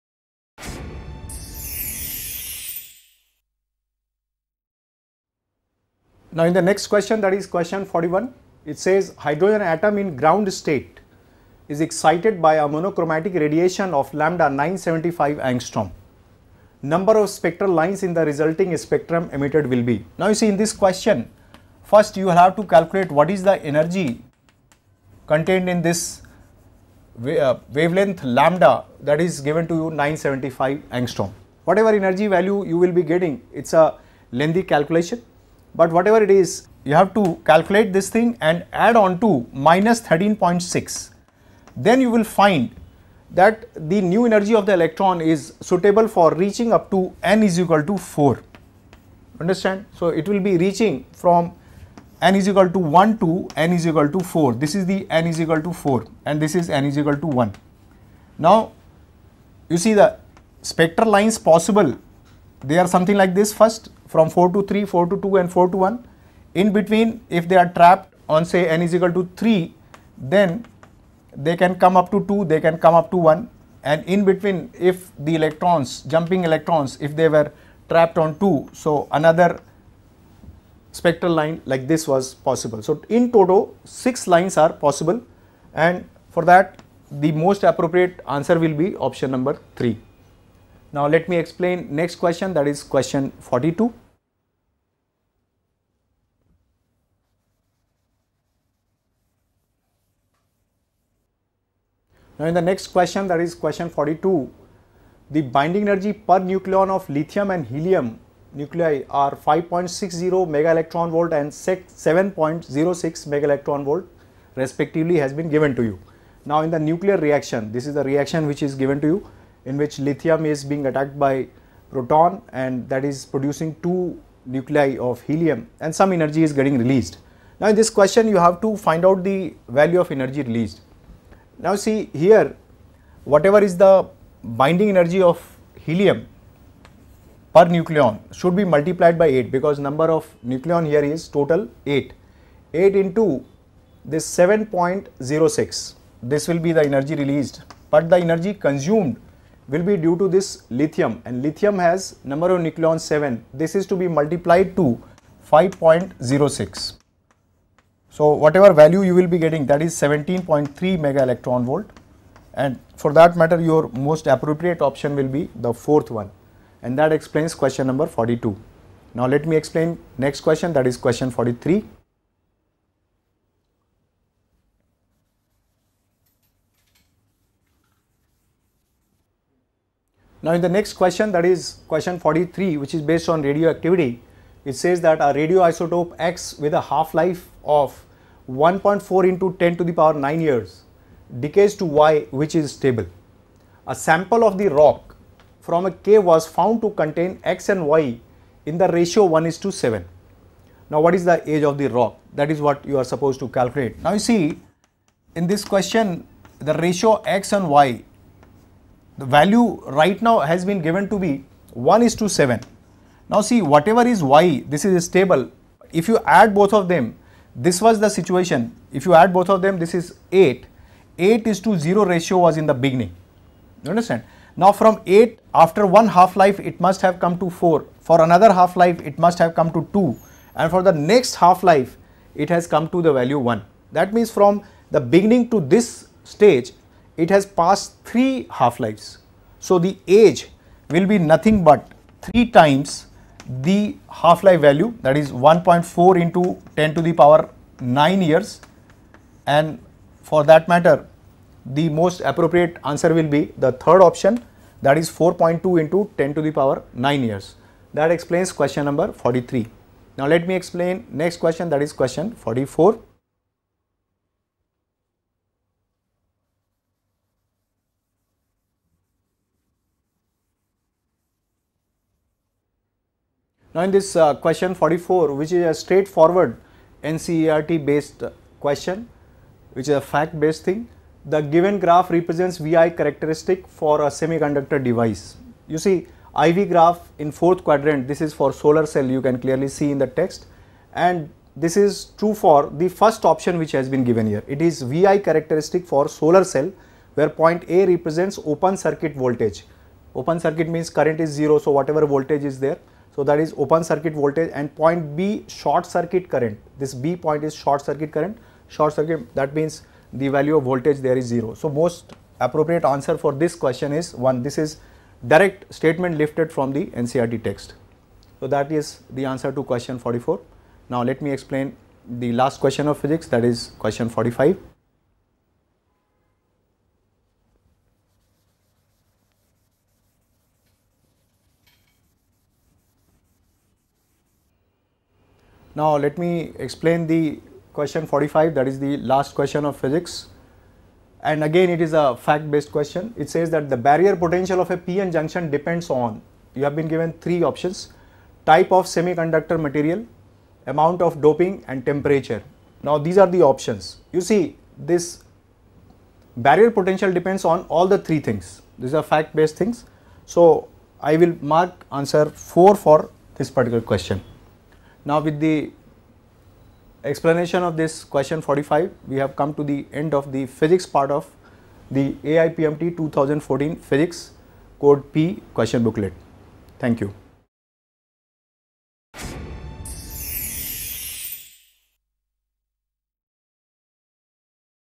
AIPMT 2014 Solutions-Physics Paper [Q-41 to Q-45] By Aakash Institute
Find AIPMT 2014 Video Solutions of Physics Paper from Question No. 41-to-45 (Code-P) by Aakash Institute. [Apply here for AIPMT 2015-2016 Courses